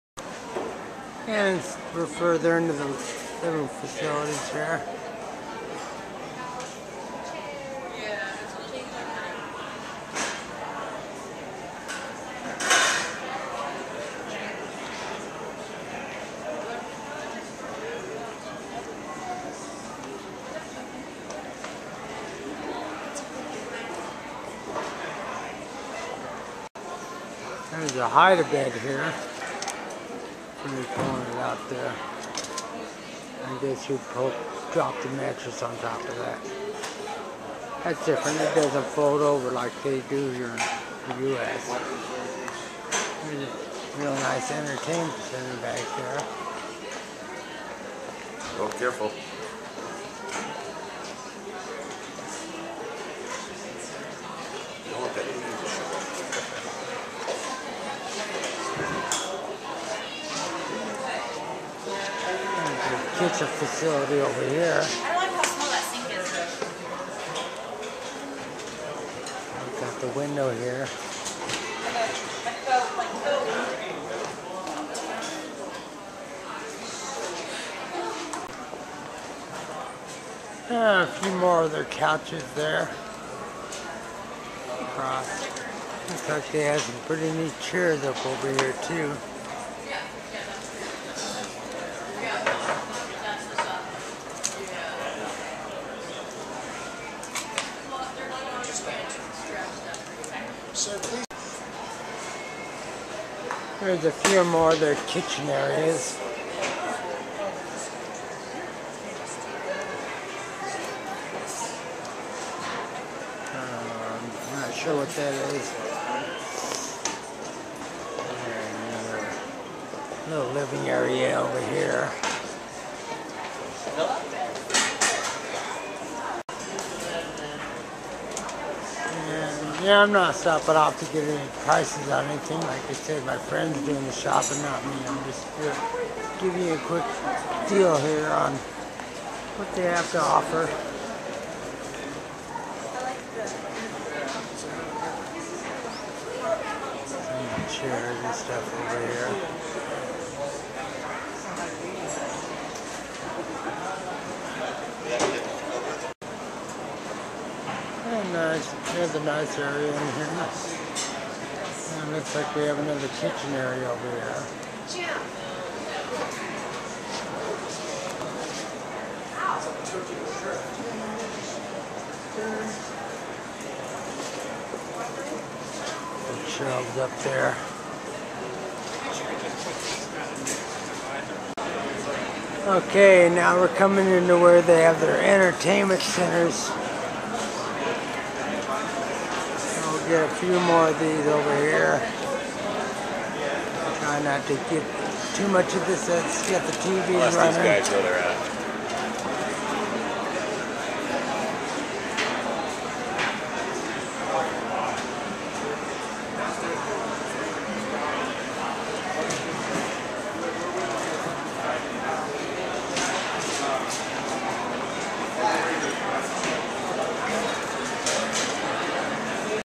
Kiev Visits IKEA #4
Well it looks like Kiev spends most of this IKEA video in the furnisher section checking out sofas & chairs. He does show a couple of Kitchen & Living-room displays. What's that one guys problem? ;=0 We hope this puts a Smile on your face and a little Sunshine in your day. :=) Chickbit & :-) Kiev